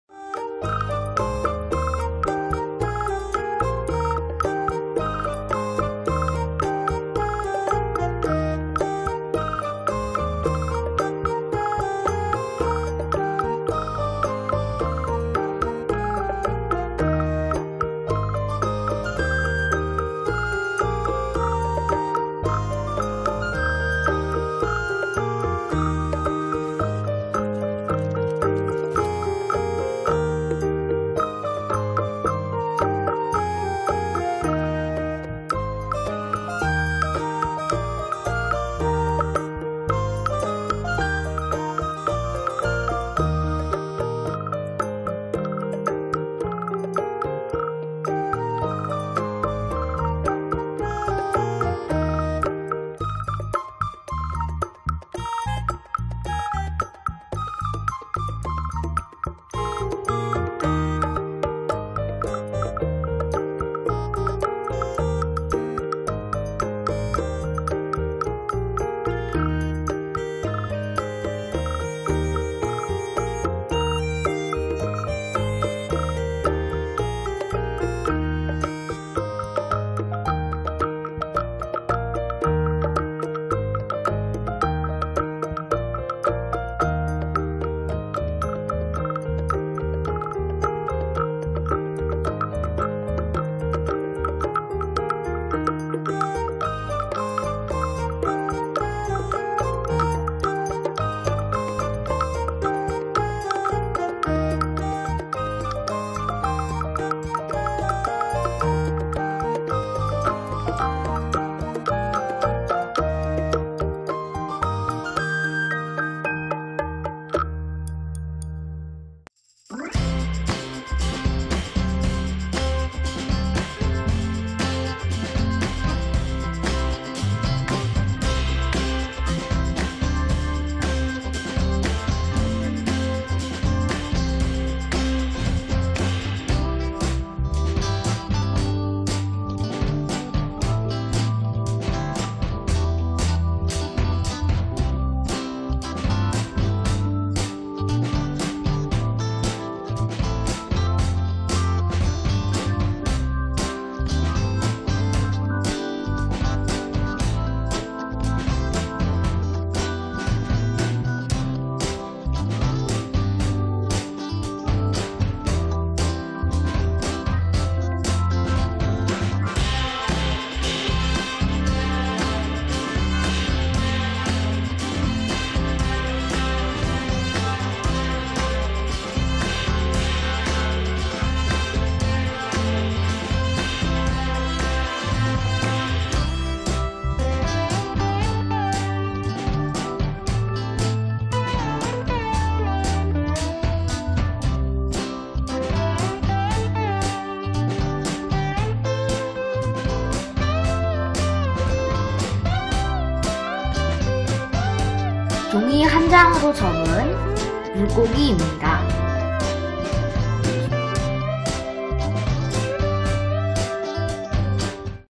물총고기 Archer Fish 종이접기 - 오리가미 아쿠아리움
오리가미 아쿠아리움에 수록되어 있는 물총고기 (Archer Fish)를 접어 봤어요.
검정색 티슈페이퍼와 종이호일을 붙여 만든 합지로 접었어요.
접기는 좀 어렵지만 다 접고 나면 아주 예쁜 물고기가 됩니다.